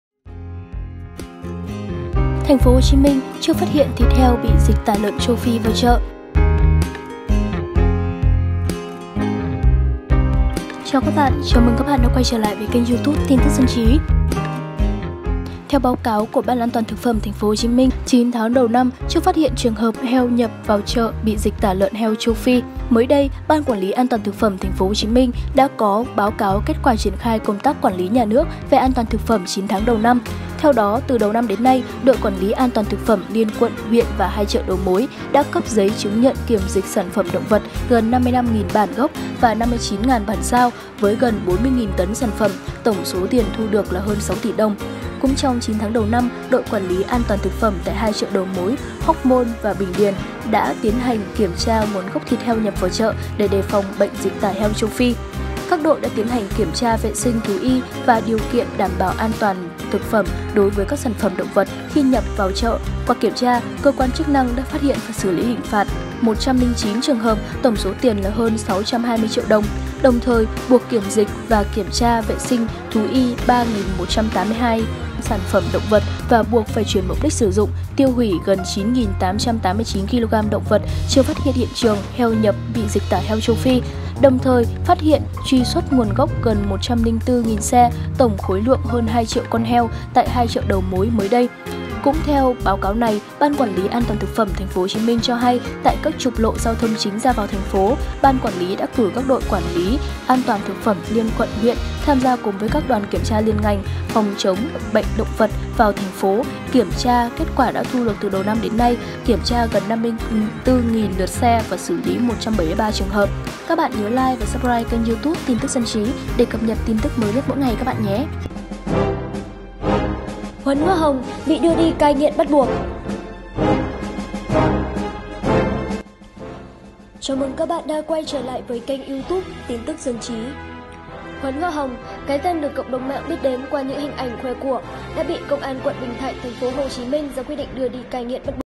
TP HCM: Chưa phát hiện thịt heo bị dịch tả châu Phi vào chợ
Theo báo cáo của Ban An toàn thực phẩm TP HCM, 9 tháng đầu năm chưa phát hiện trường hợp heo nhập chợ bị bệnh dịch tả heo châu Phi.